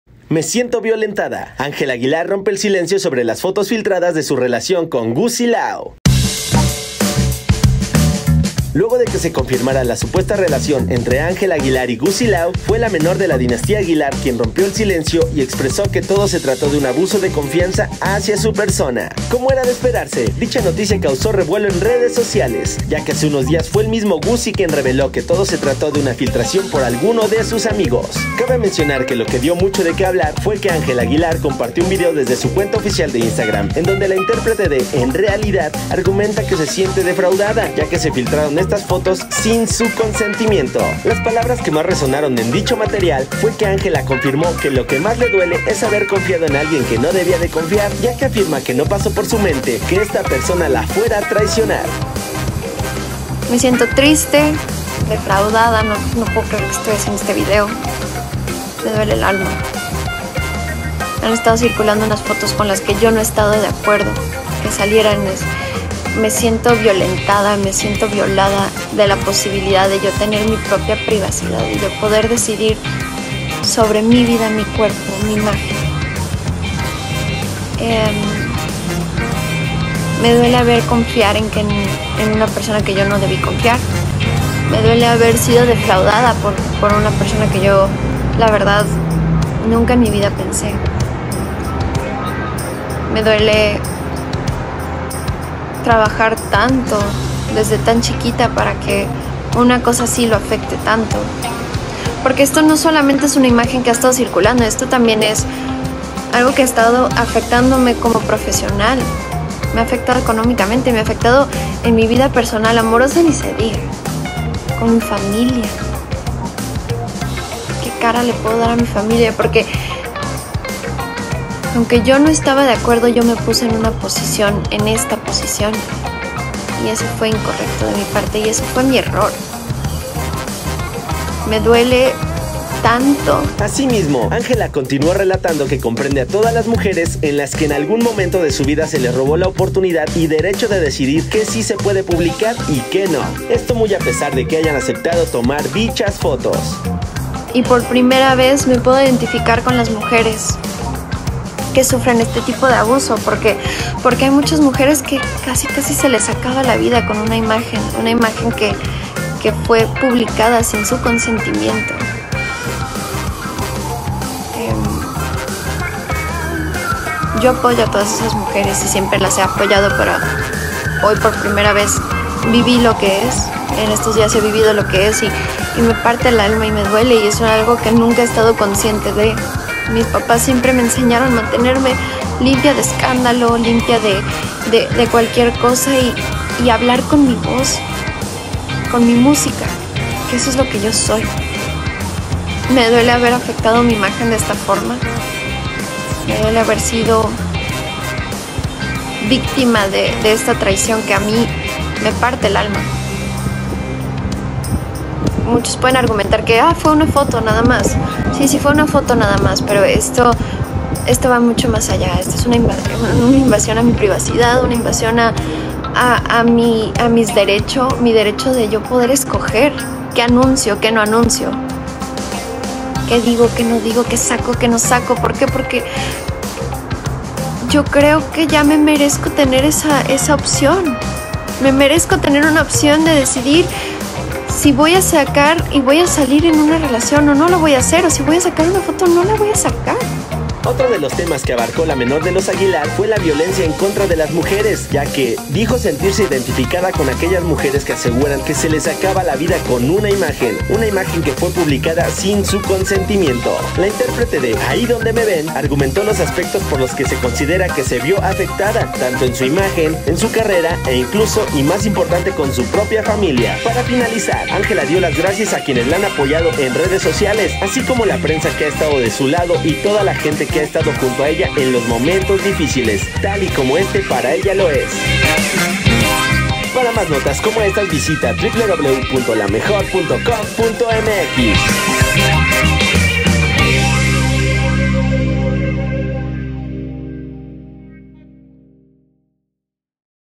“Me siento violentada” Angela Aguilar habla de las fotos filtradas con Gussy Lau
Angela asegura que esta polémica le ha afectado en su carrera profesional y personal